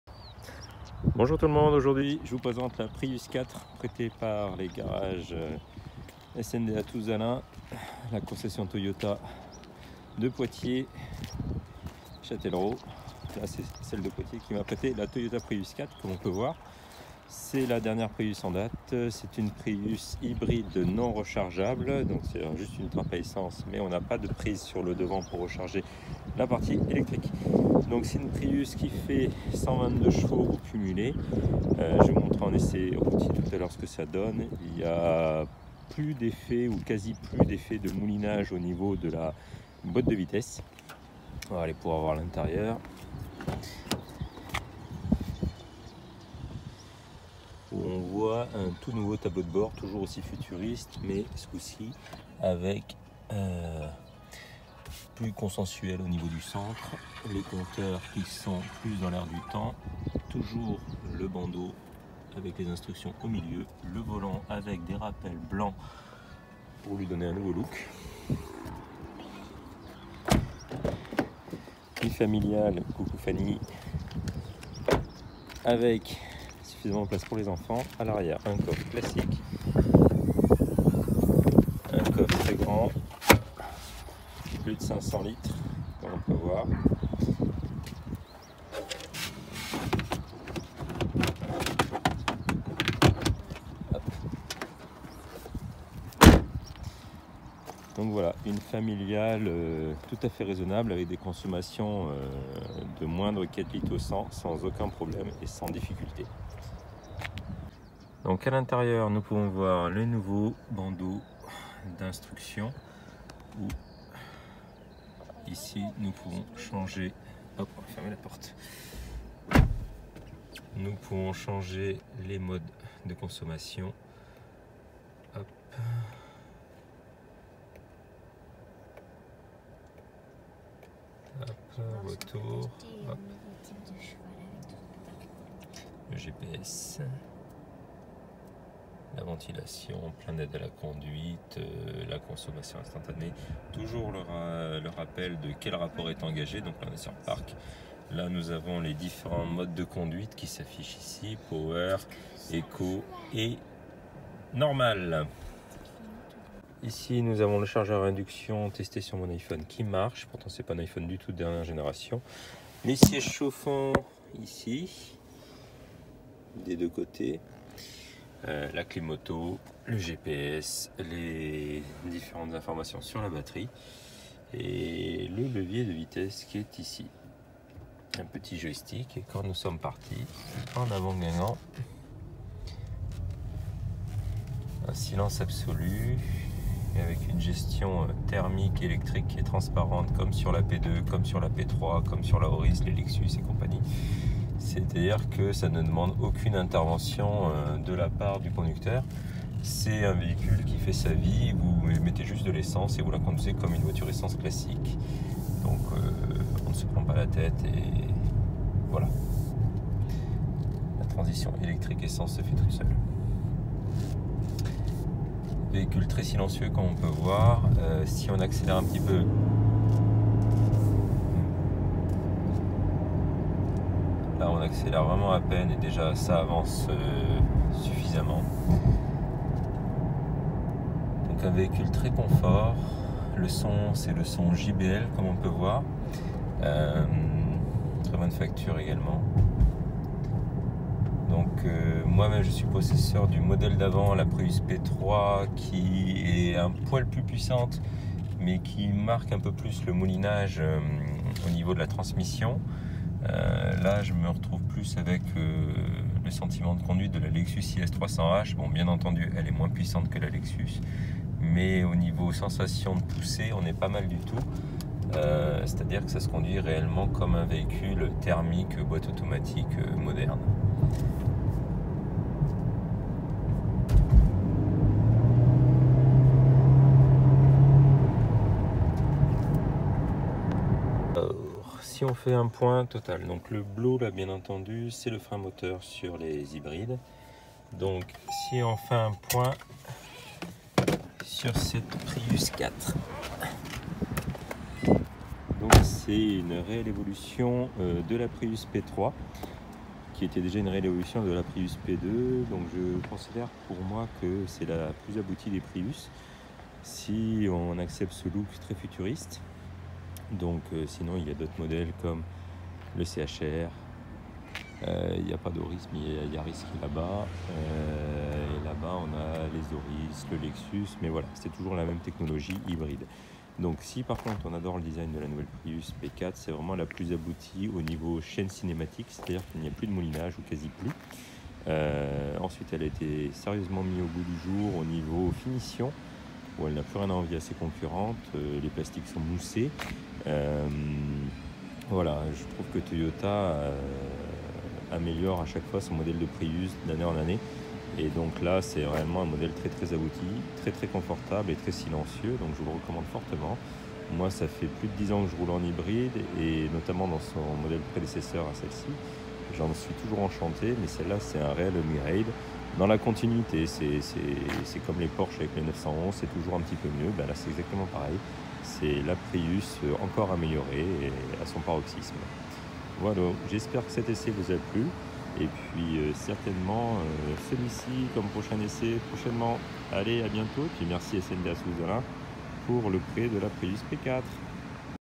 Essai Toyota Prius 4 hybrid
Merci à la concession SNDA Touzalin de Poitiers pour le prêt de la voiture.

La Toyota Prius de quatrième génération marque un progrès radical en matière d'efficacité énergétique.

La Prius repose sur une nouvelle plateforme issue de l'architecture TNGA (Toyota New Global Architecture) qui abaisse le centre de gravité au profit de la réactivité et de la stabilité du châssis.

Le système hybride de nouvelle génération de la Prius repose sur un moteur essence 1.8 litre VVT-i à cycle Atkinson et deux moteurs électriques/générateurs.

Le rendement thermique du moteur essence 1.8 litre VVT-i à cycle Atkinson dépasse 40% grâce à des modifications du moteur visant à diminuer les frottements et les pertes mécaniques.

La quatrième génération de la Toyota Prius marque un progrès important en consommation, avec des émissions de dioxyde de carbone de 75 g/km, soit une consommation en cycle mixte d'environ 3,4 litres aux 100 km.

Le système hybride de la Toyota Prius de quatrième génération développe une puissance maximale cumulée de 122 ch (98 ch pour le moteur thermique et 72 ch pour les deux moteurs électriques d'entraînement).

Le moteur essence délivre un couple maximal de 142 Nm à 3 600 tr/min.

Le moteur électrique d'entraînement fournit un couple de 163 Nm.